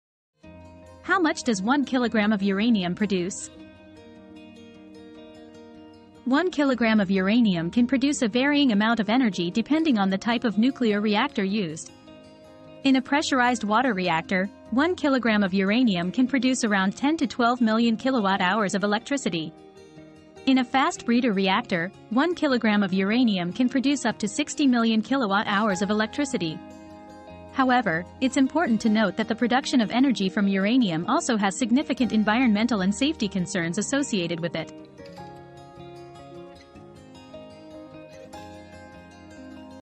How much does 1kg of uranium produce?
Get ready to learn how much energy 1kg of uranium can produce! Depending on the type of nuclear reactor used, the amount of energy can vary greatly. In this video, we'll explore the different amounts of electricity 1kg of uranium can generate in pressurized water reactors and fast breeder reactors. But, we'll also take a closer look at the environmental and safety concerns surrounding this energy source. Don't miss out on this fascinating exploration of nuclear energy!